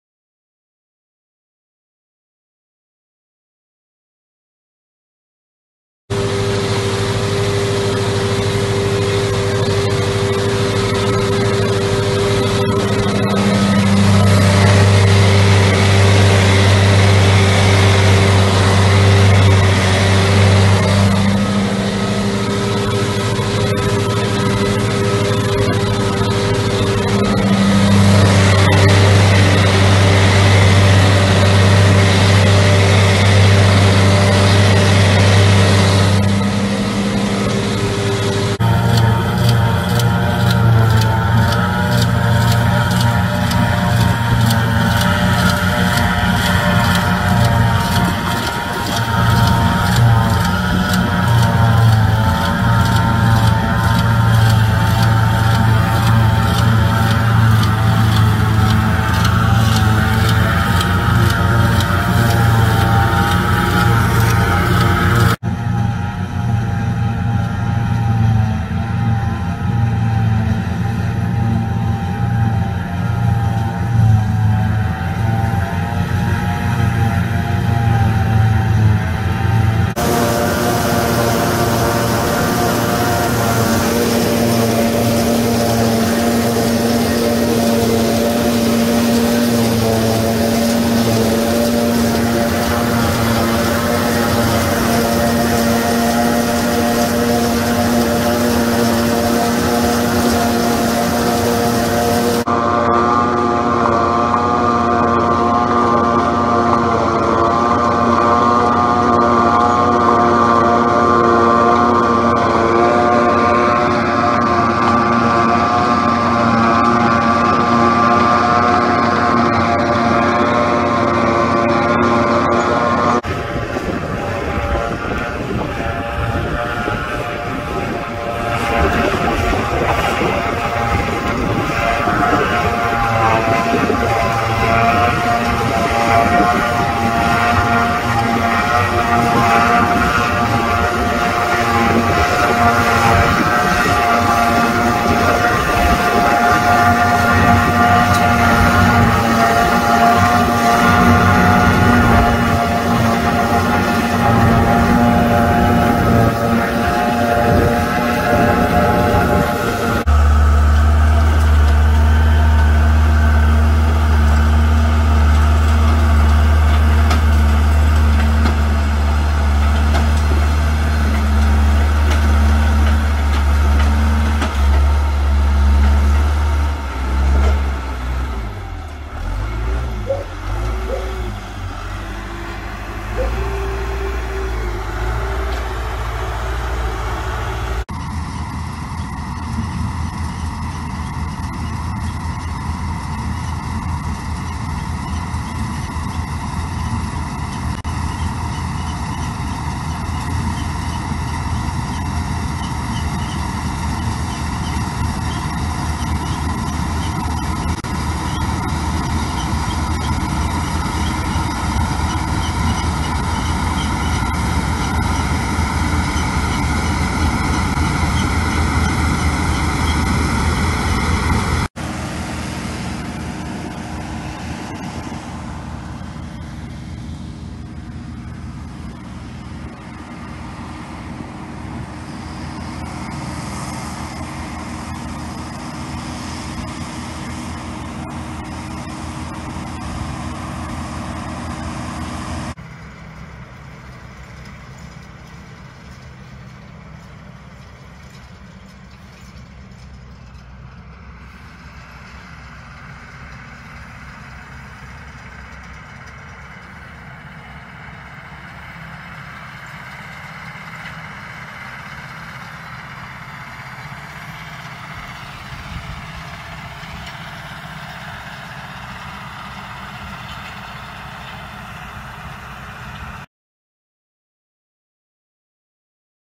Old-School Classics Silage, Grain Summer 2021. Hesston 7725, Deutz-Fahr 2680, 910, Ford, new Holland
Some clips from summer 2021 with classics at work at Silage and Barley/Grain in West Cork, Ireland.

Hesston 7725, Ford, New Holland tractors, Cat / Caterpillar 910 on the pit, Deutz-Fahr 2680 at barley and a Valtra & Fusion included for good measure.

Some clips were uploaded previously.